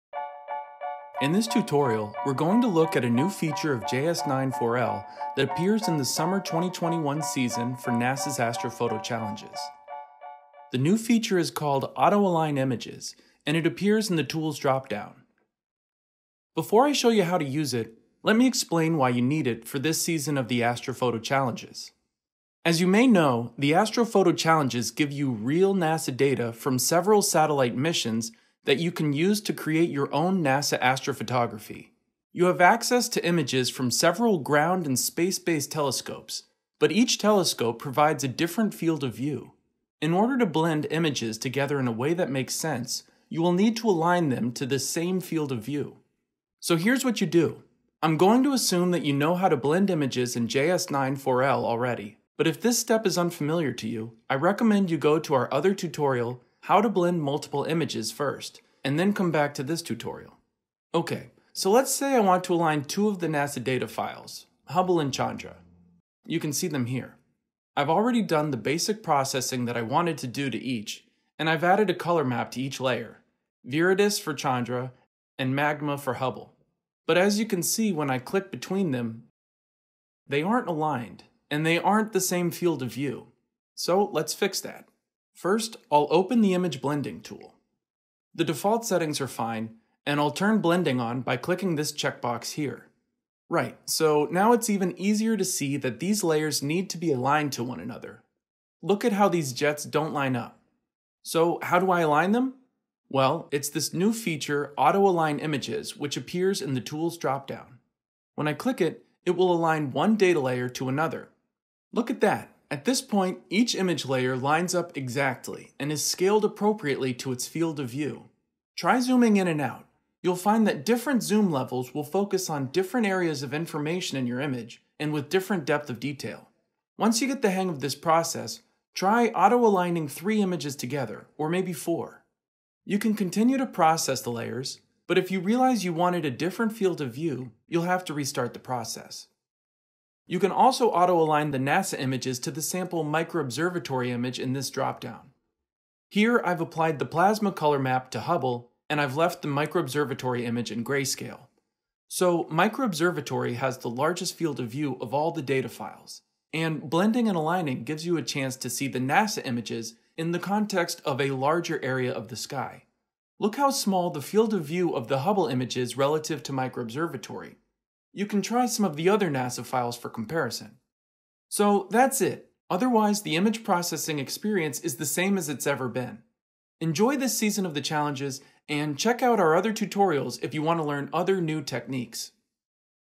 NASA's Astrophoto Challenges | How to Auto-Align Images
This video tutorial will demonstrate how to auto-align two or more NASA and/or MicroObservatory images with different fields of view for NASA's Astrophoto Challenges.

This tutorial uses the JS9-4L image-processing software included with the MicroObservatory Robotic Telescope Network.